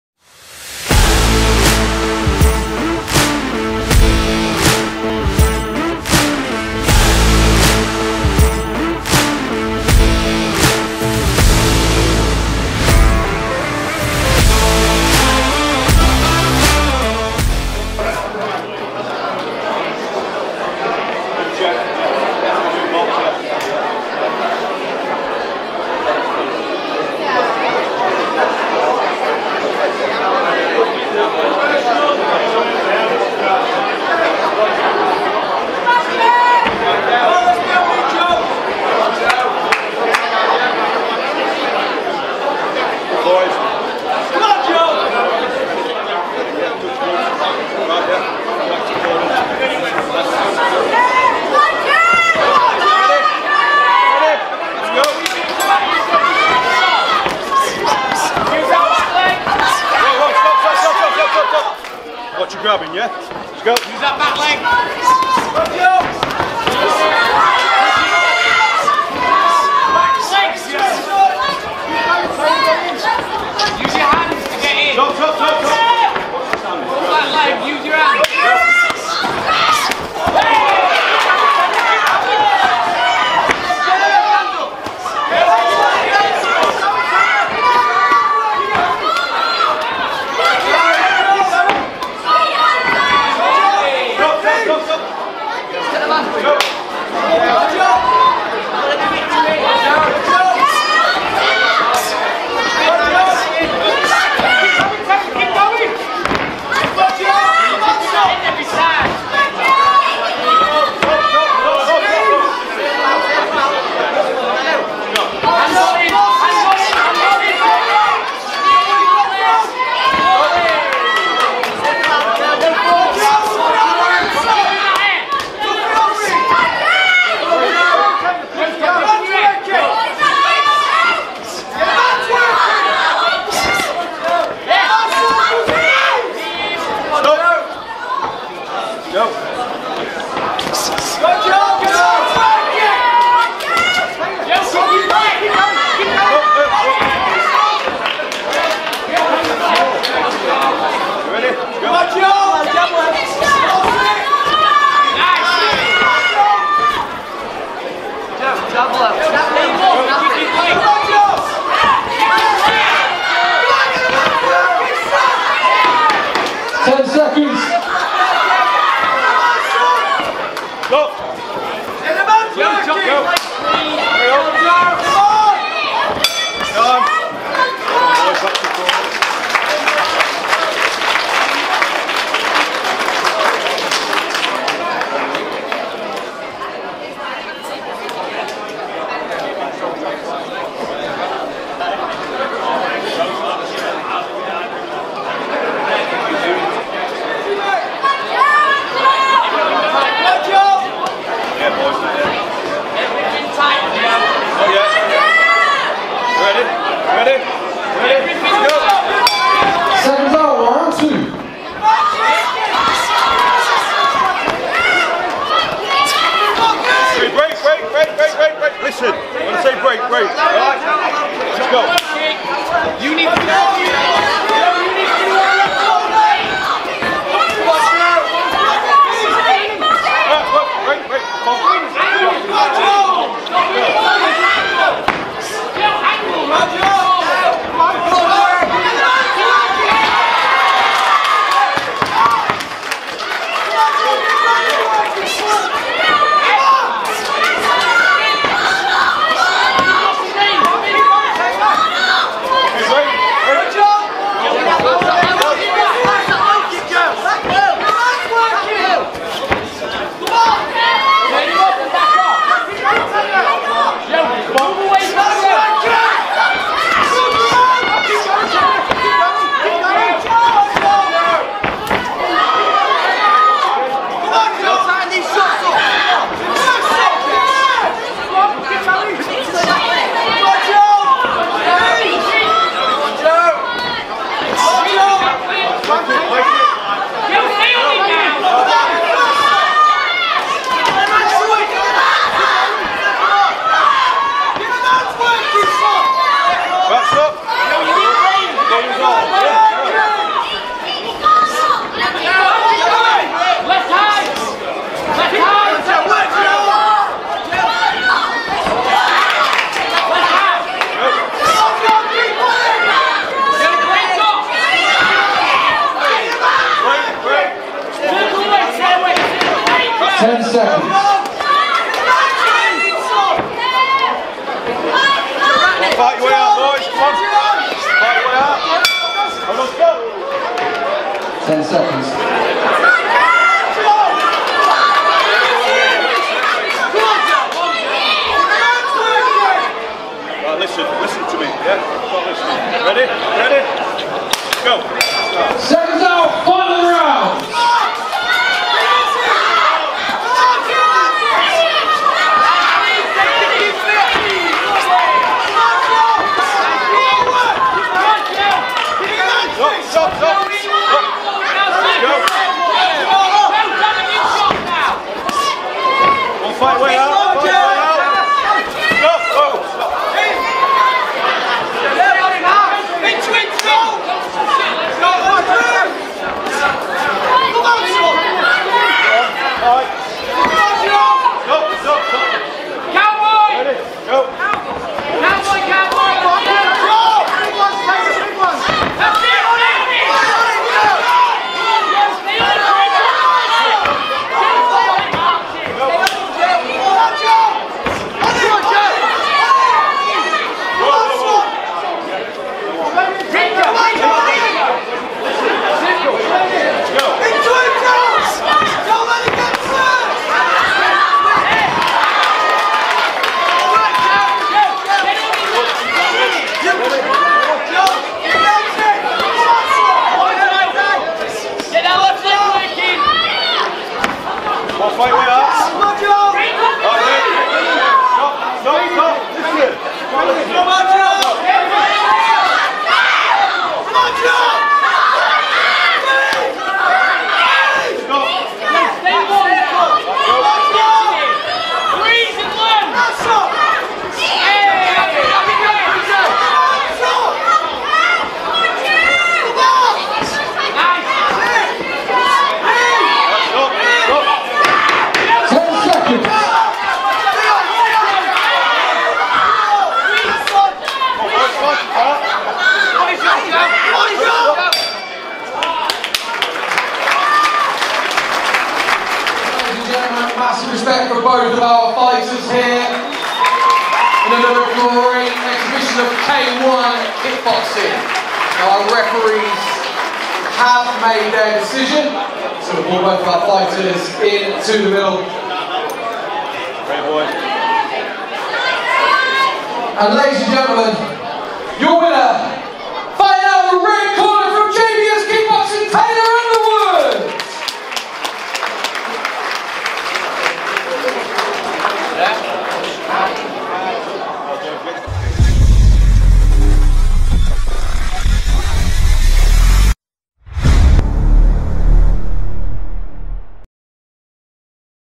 28 May 2022 Ring Warriors Joe Cape vs Taylor Underwood
RECC Ring Warriors

Joe Cape representing Unit MMA

Taylor Underwood representing JBS Kickboxing